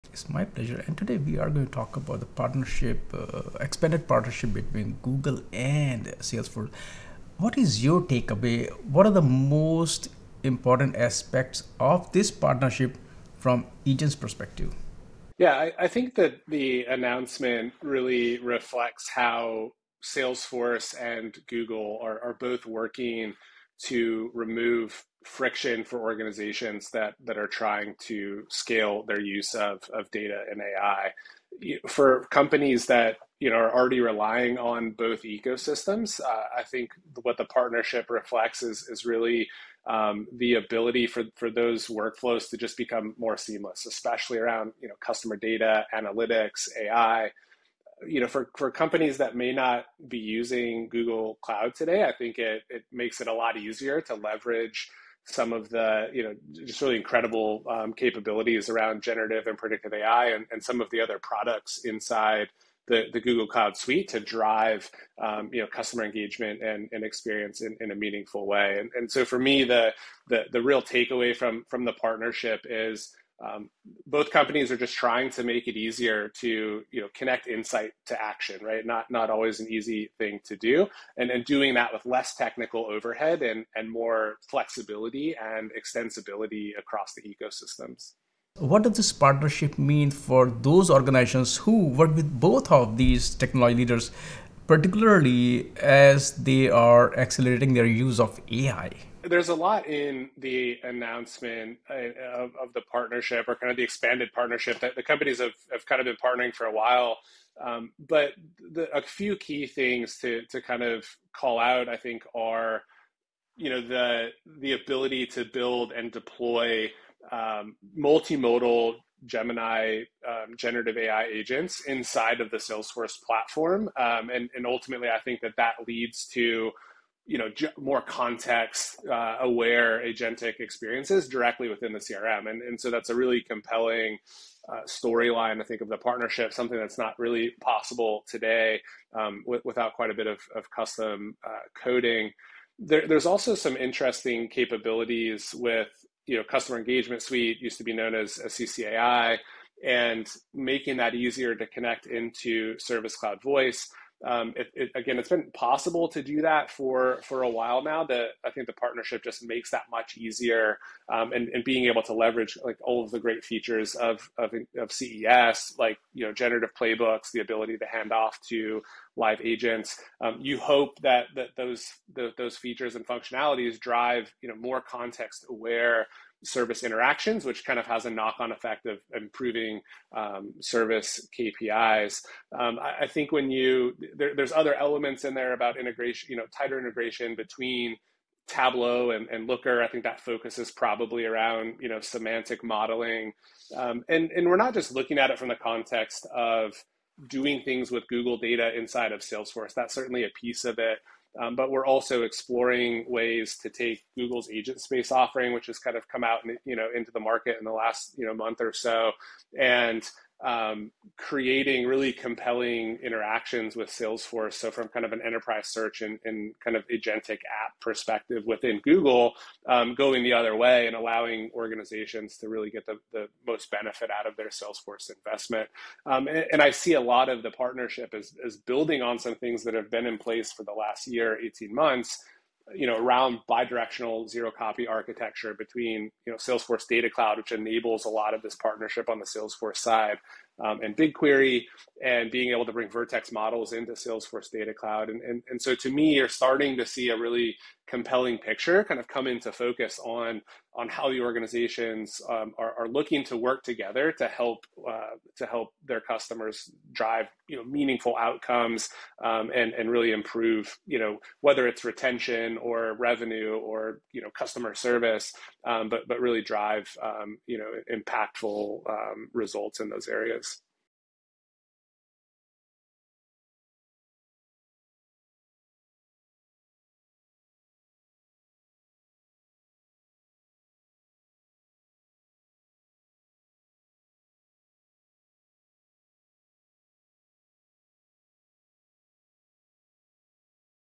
How Google & Salesforce Are Redefining Enterprise AI with Agentic Experiences
In this episode of An Eye On AI, host Swapnil Bhartiya talks with Ian Hagerman, Principal Architect at Egen, about the implications of the expanded partnership between Google Cloud and Salesforce. Hagerman dives deep into how this collaboration removes friction for organizations scaling their AI and data operations, particularly by enabling seamless, context-aware workflows across platforms. He explains how multimodal Gemini agents, zero-copy architecture, and integration with tools like Looker and Tableau empower businesses to drive real-time insights and improve customer engagement.

Tune in to learn how this partnership simplifies AI deployment across ecosystems, bridges Salesforce Data Cloud with Google BigQuery, and enhances service KPIs with features like generative playbooks and intelligent handoffs.

Key Topics Covered:
Multimodal Gemini AI agents in Salesforce platform
Customer Engagement Suite integration with Service Cloud Voice
Bi-directional zero-copy architecture between Salesforce Data Cloud and BigQuery
Vertex AI models integration with Salesforce ecosystem
Enterprise search and agentic applications impact
Guest: Ian Hagerman, Principal Architect at Egen (egen.ai) Host: Swapnil Bhartiya, TFiR